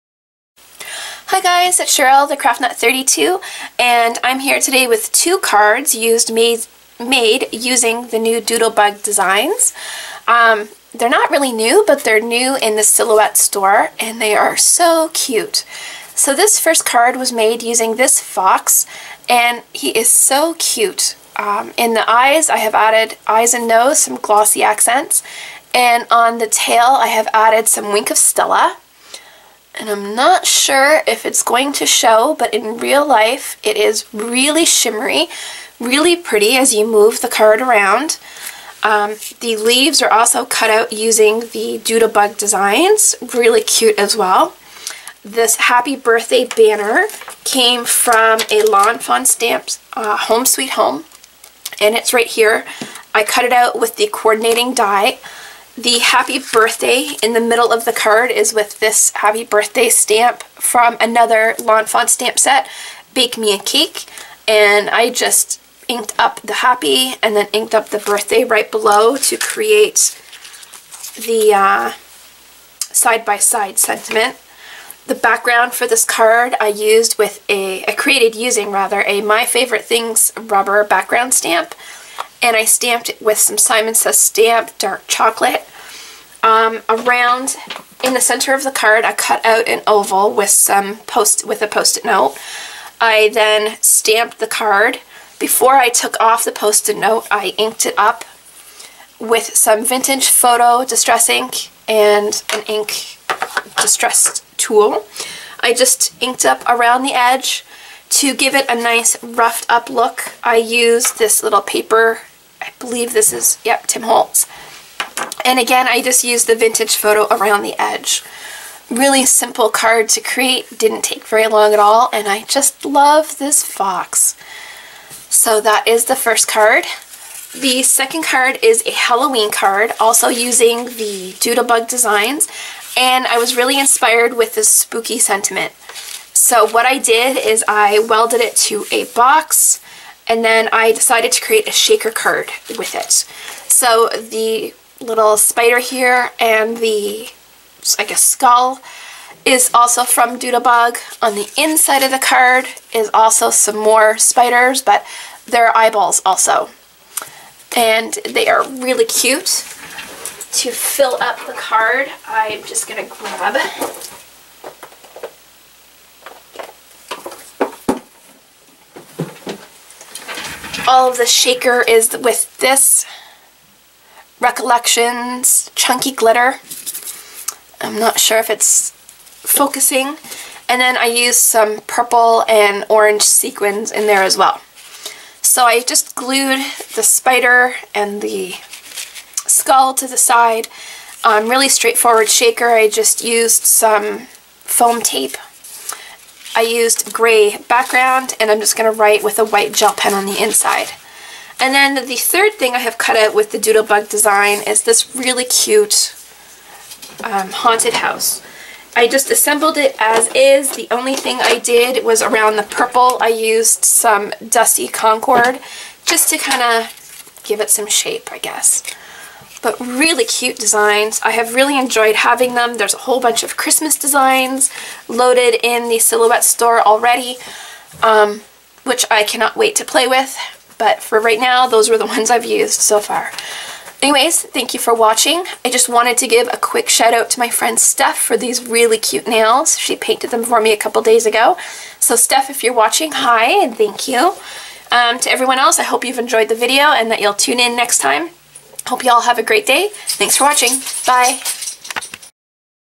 Fun With Doodlebug Designs
Two fun cards created using the cute new Doodlebug designs from the Silhouette Store